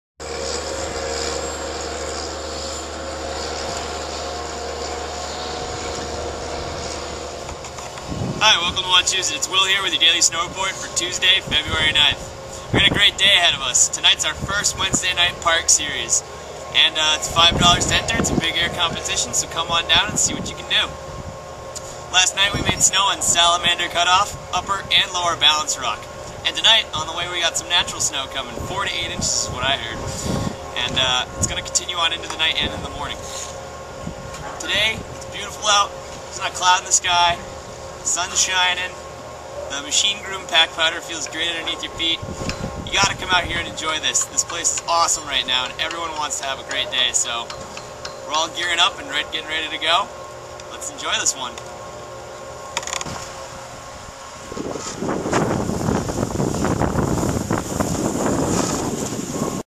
Feb. 9th Wachusett Mountain Daily Video Snow Report
The conditions are great! we just made snow last night on Salamander cutoff and upper+lower balance rock. Fresh natural snow on its way all day tomorrow!